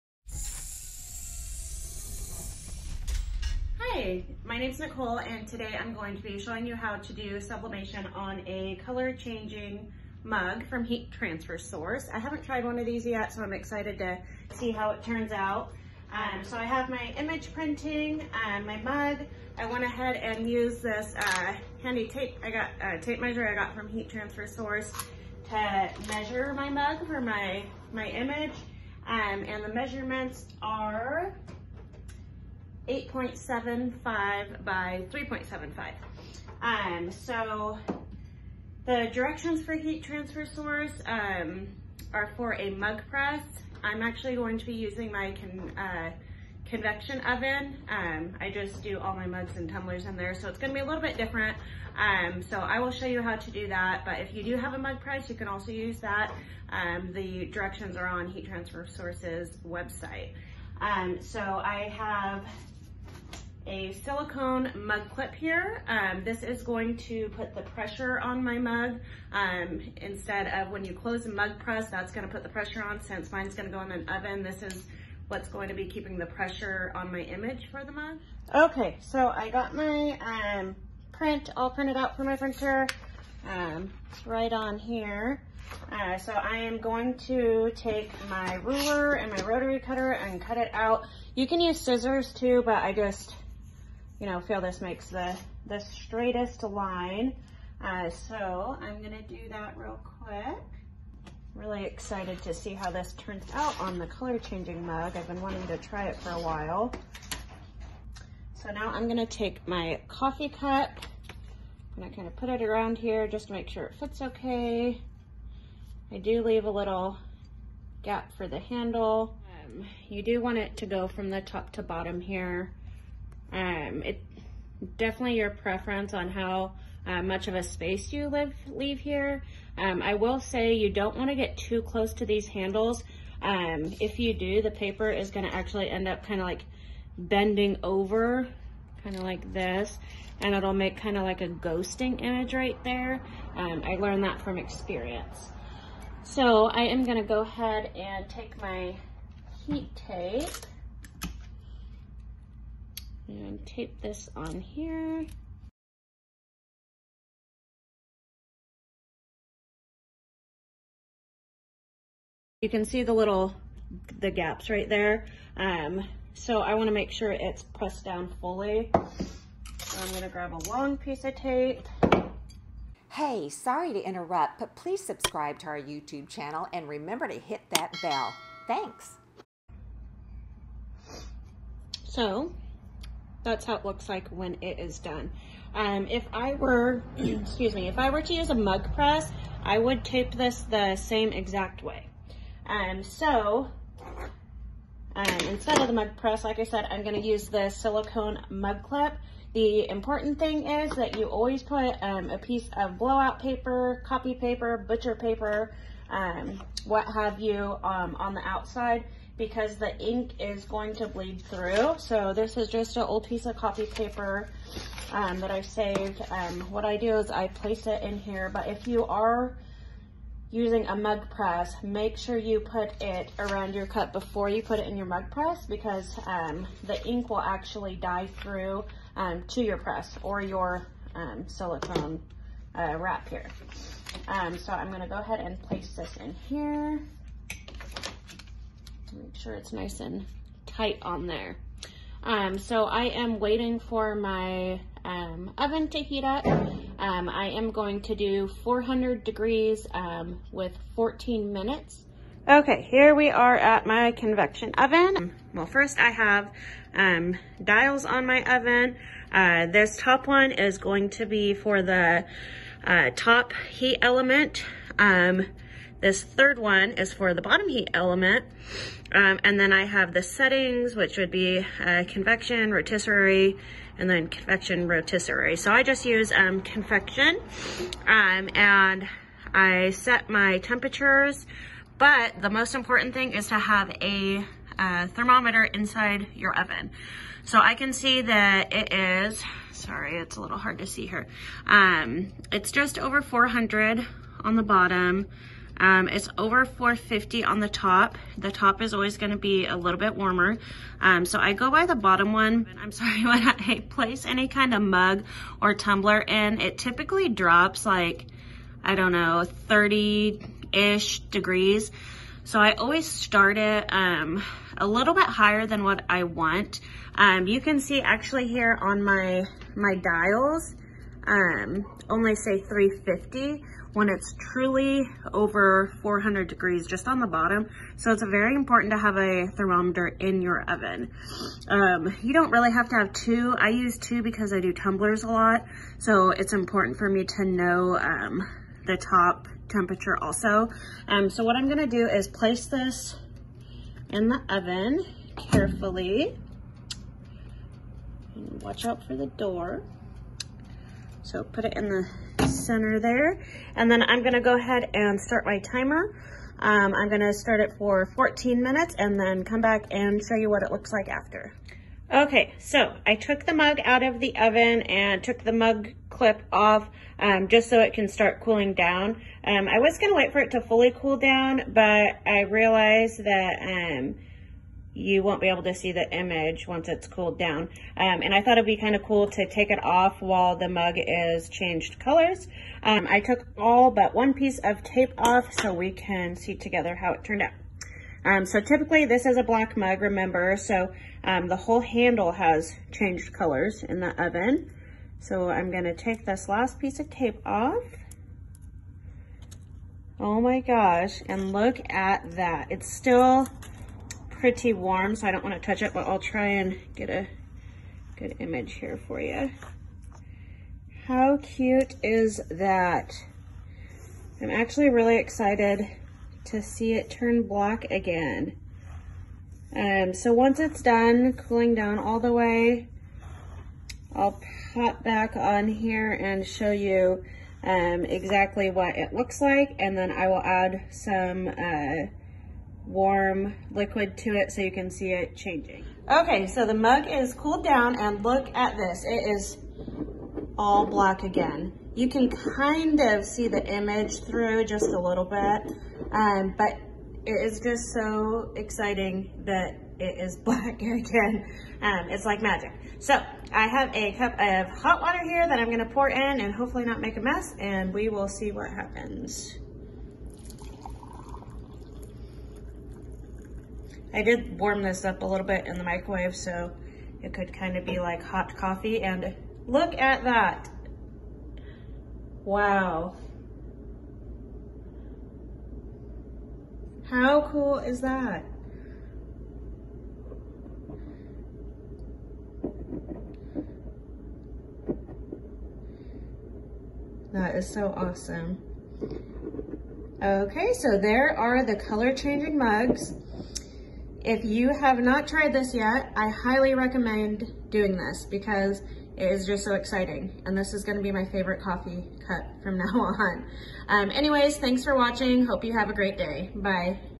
COLOR CHANGING MUG SUBLIMATION
Watch Nicole make a cool Color Changing Mug. They’re so easy you’ll probably want to make several as gifts, too.

👩🏻‍💻 Join our mailing list to get First Dibs on incredible sales! 3qEJOp7